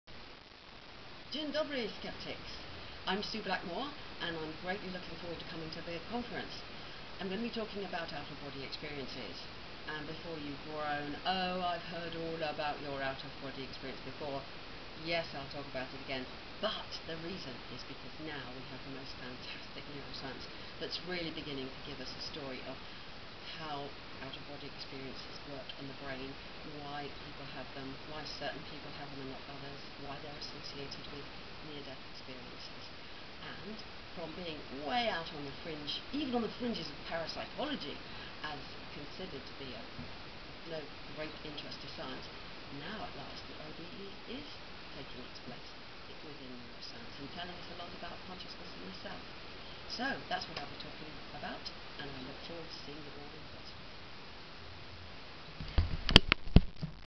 ESC 2017 - Susan Blackmore
Susan Blackmore invites you to attend the 17th European Skeptics Congress and hear her talk: “Positive Scepticism: The New Science of Out-of-Body Experiences."